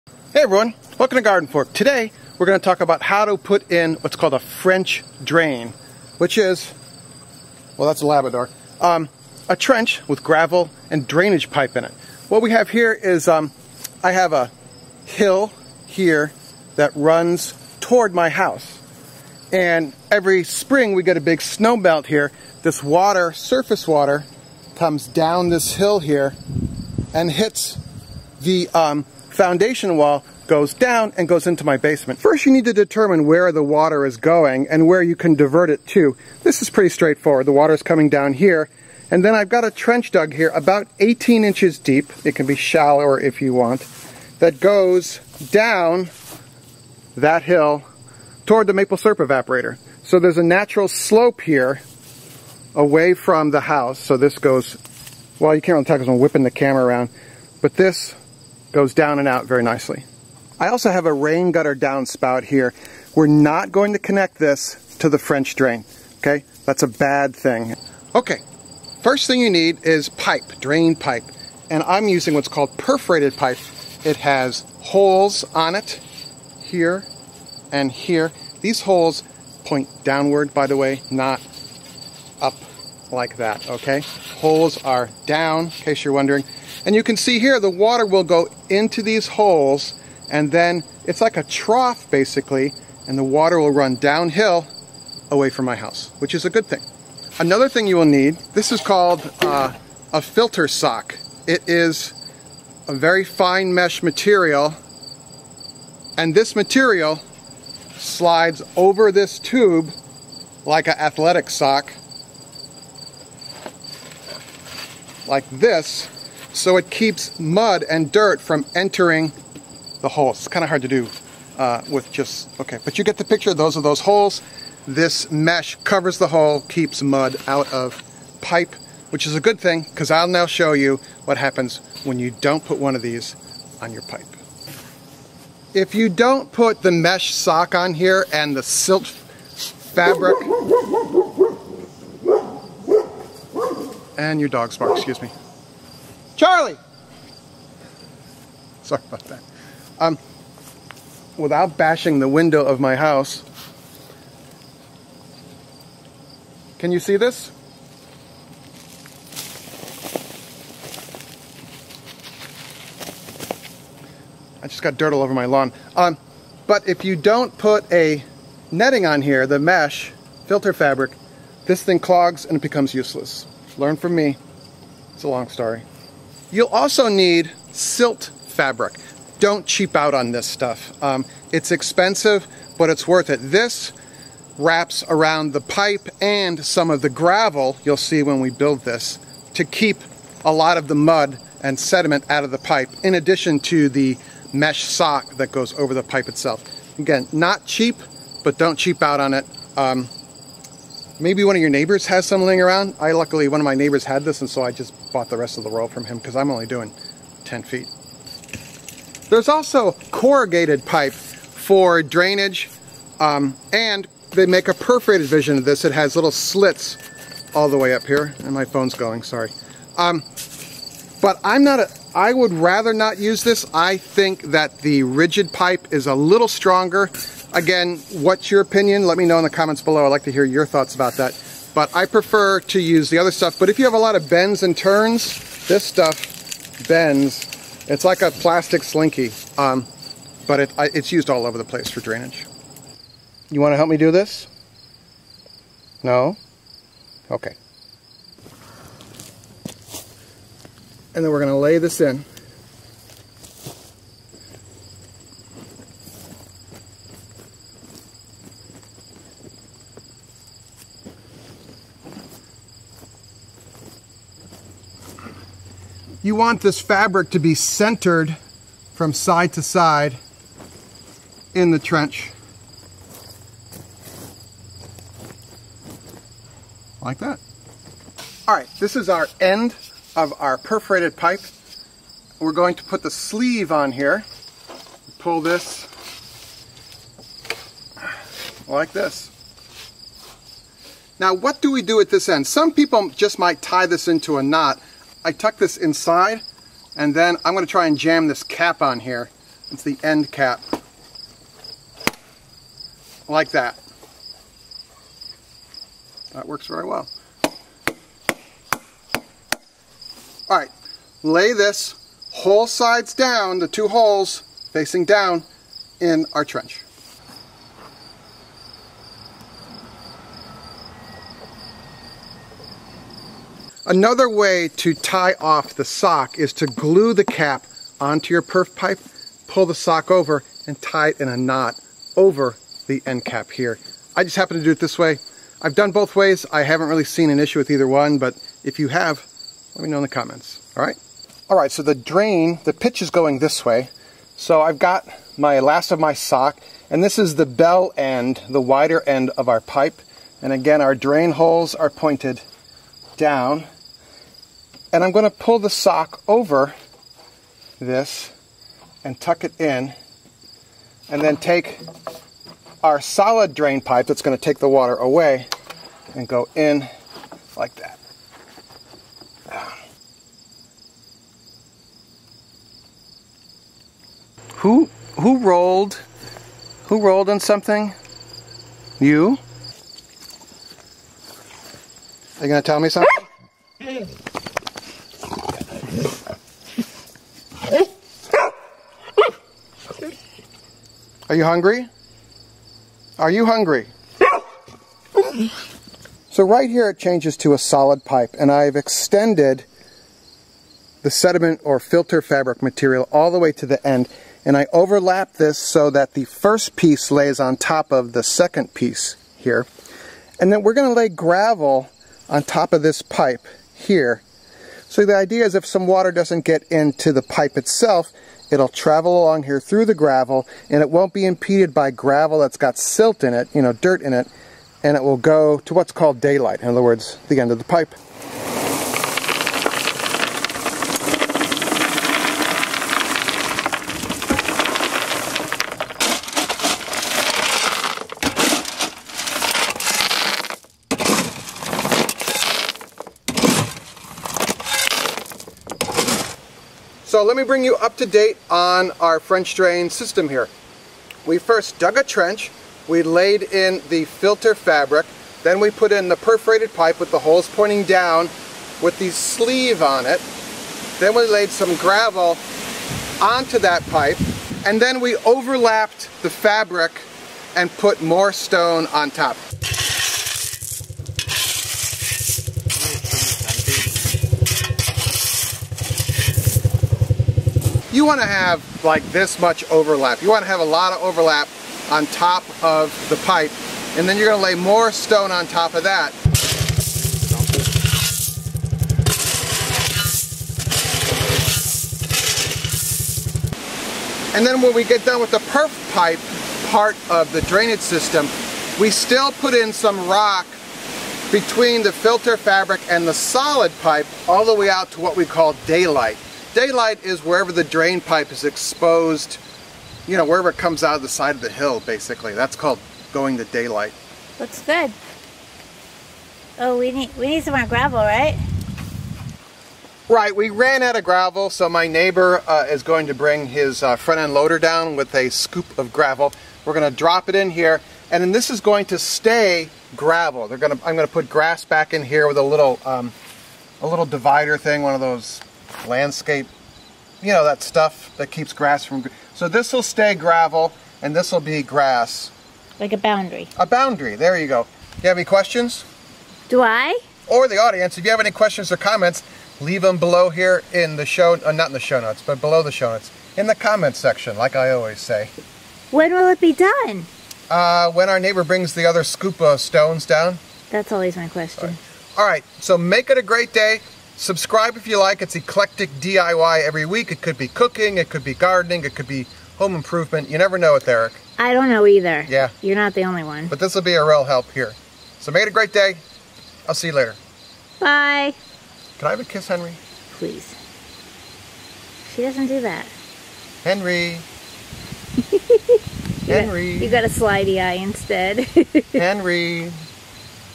How To Put In A French Drain - GardenFork
Install a French Drain Yourself with this DIY video. For backyard drainage or water leaking into the basement, this is the first line of defense. Basement Crack Vid